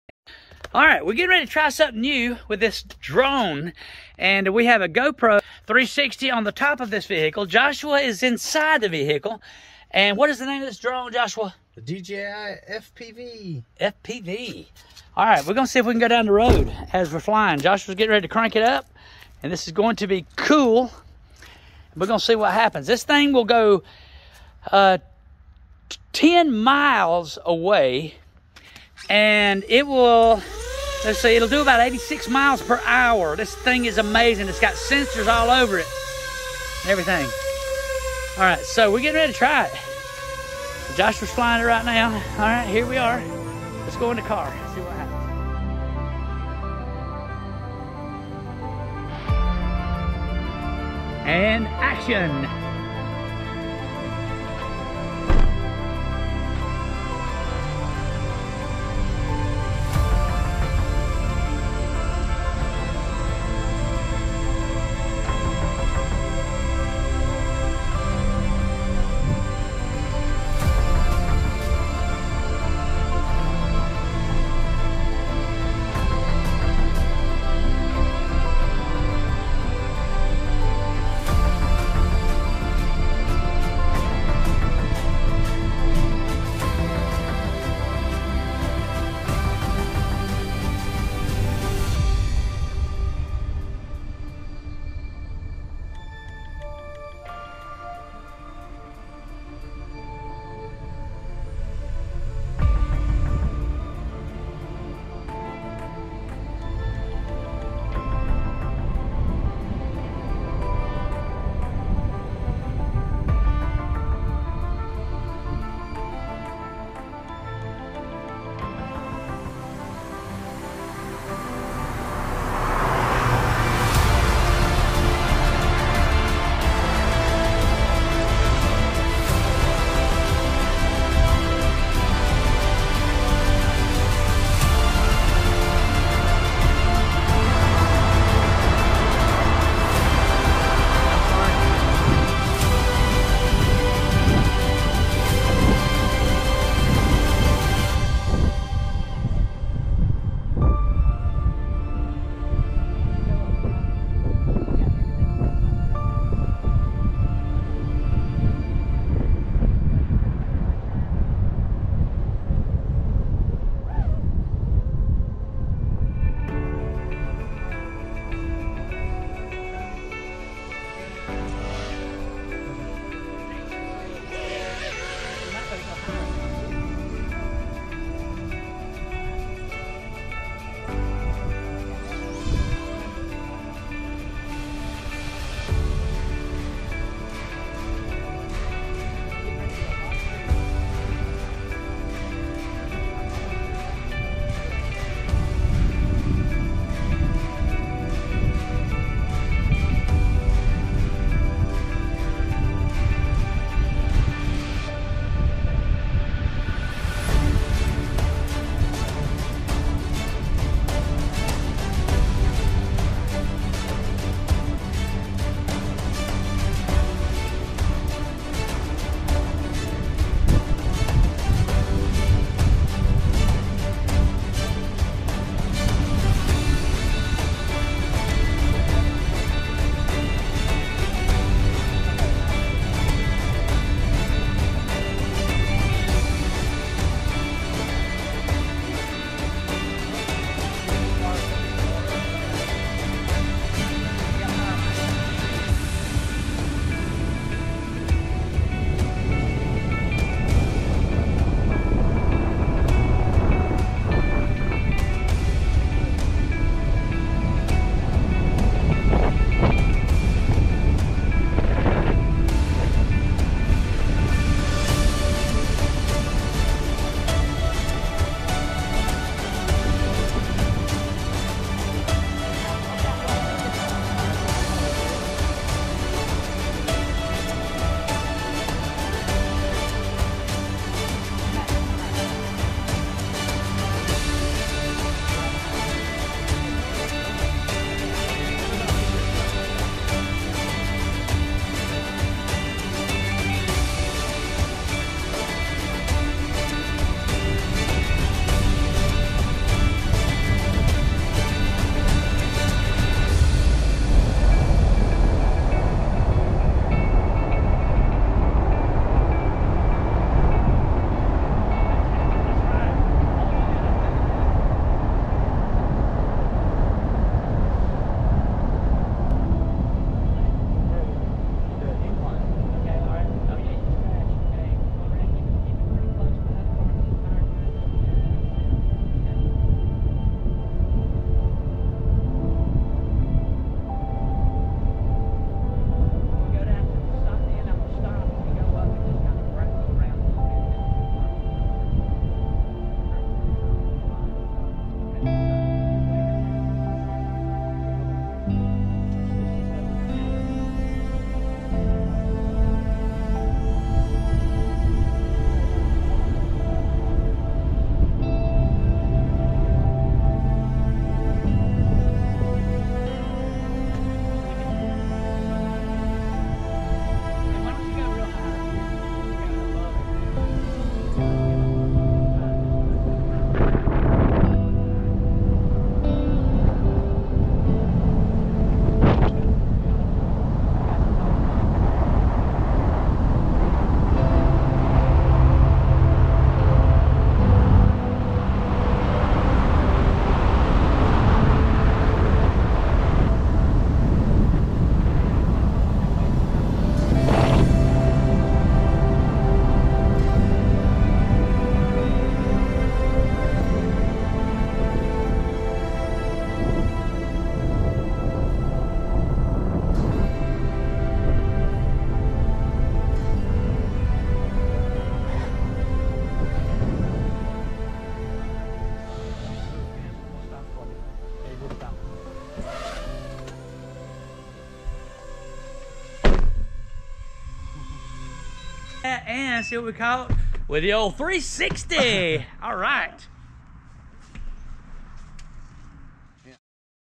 Lexus Drone Chase Joshua Hamby Pilot
FPV Drone Joshua Hamby and Daniel Hamby, Drone Chasing Lexus SUV